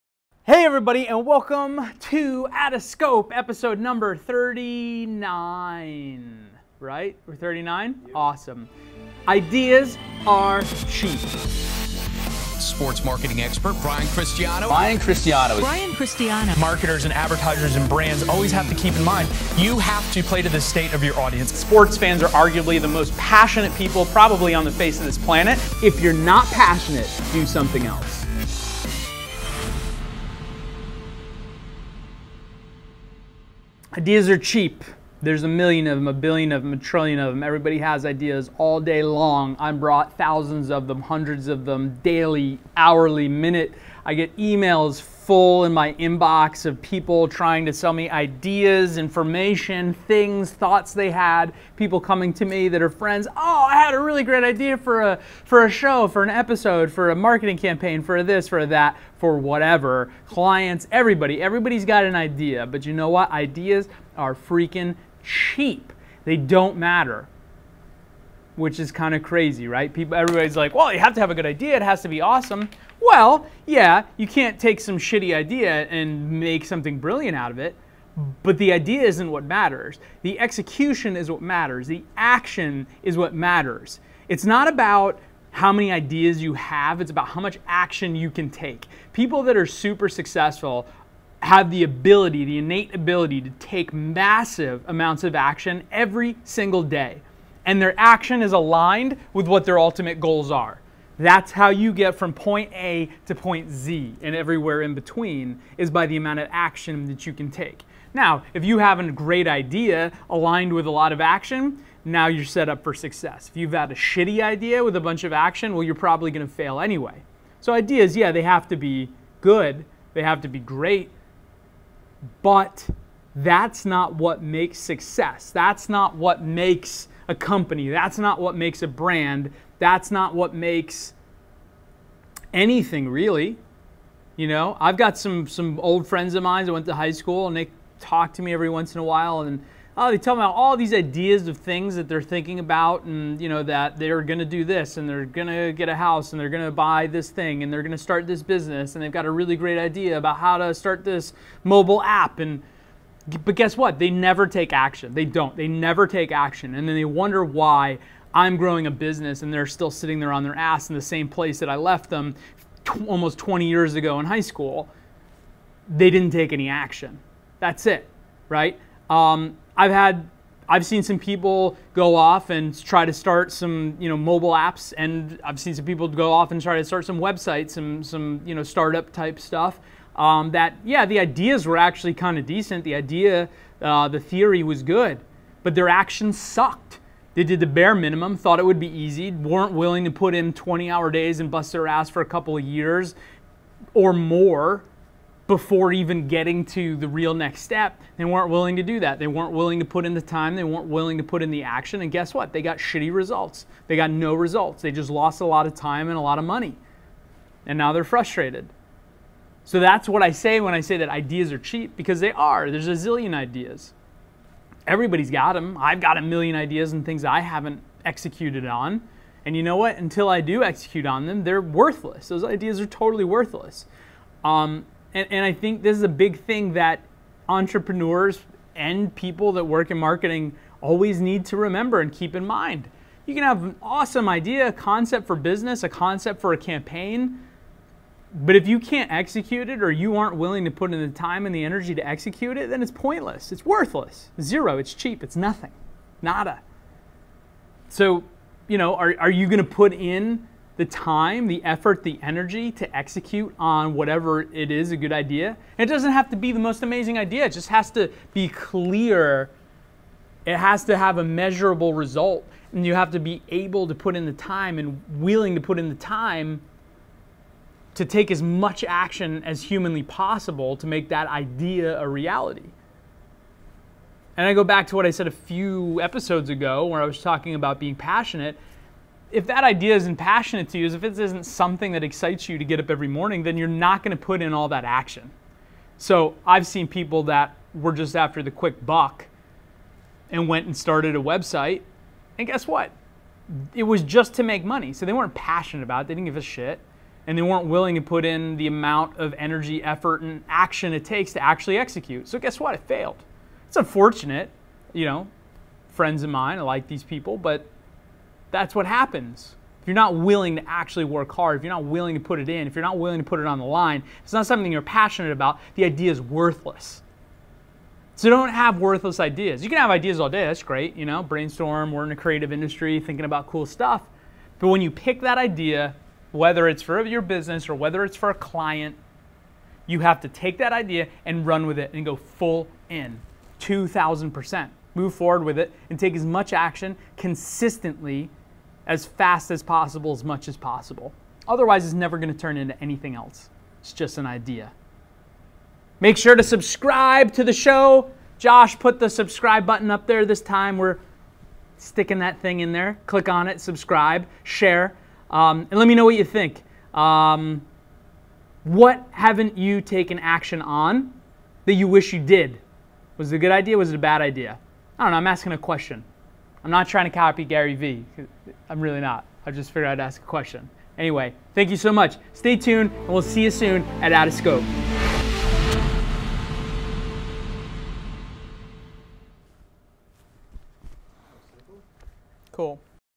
OUTOFSCOPE Episode 39: Ideas are Cheap!
Brian Cristiano, CEO of BOLD Worldwide shares his thoughts on why ideas are insignificant to a business…at least most of them; however, he shares that it’s what you do with those ideas that defines their level of importance (1:21).  Finally, he points out that in order to achieve your goals, your ideas must have clear, measurable results (3:52).